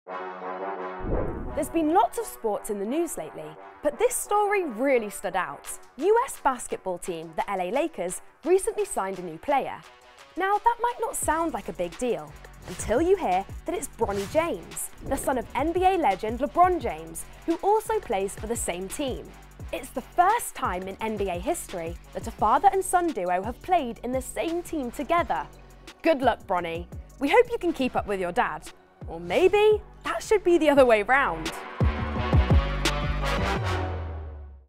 FYI: LeBron James and son Bronny make history
Basketball legend LeBron James and his son Bronny have made NBA history as the first father and son duo to play in the same team!